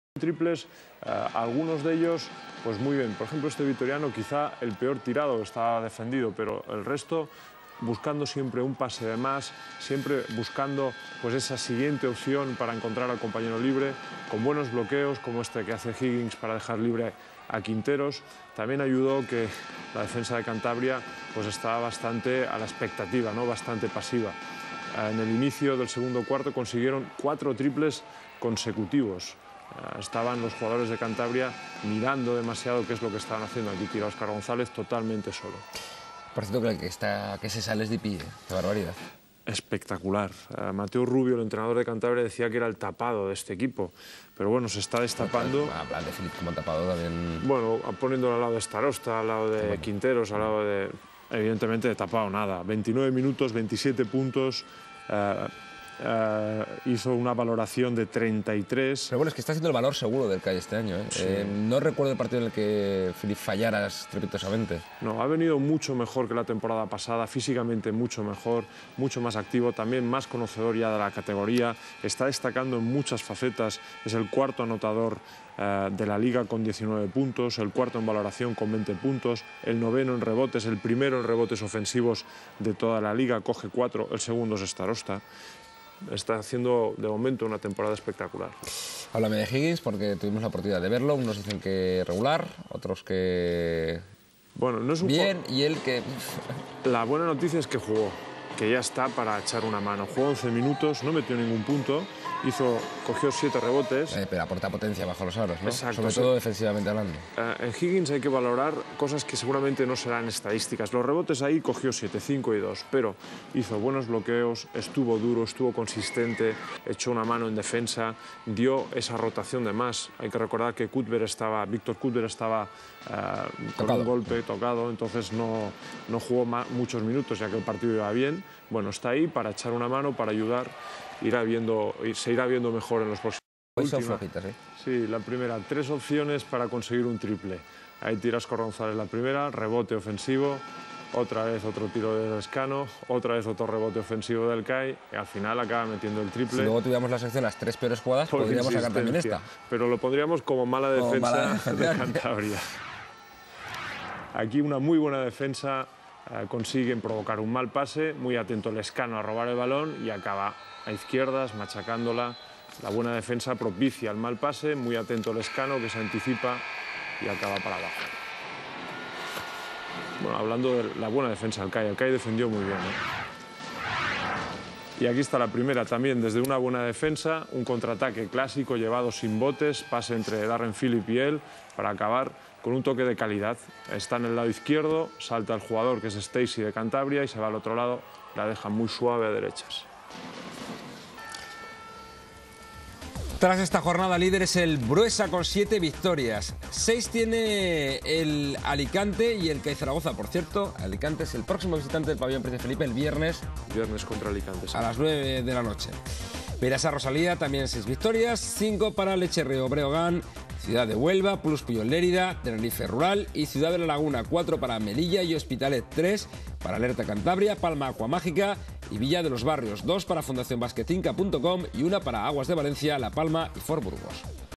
LEB Oro J8 Alerta Cantabria 75 CAI Zaragoza 91
Resumen del partido de la Jornada 8 de LEB Oro temporada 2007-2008 entre el Alerta Cantabria Lobos y el CAI Zaragoza, emitido por Aragón Televisión con los comentarios de Pep Cargol.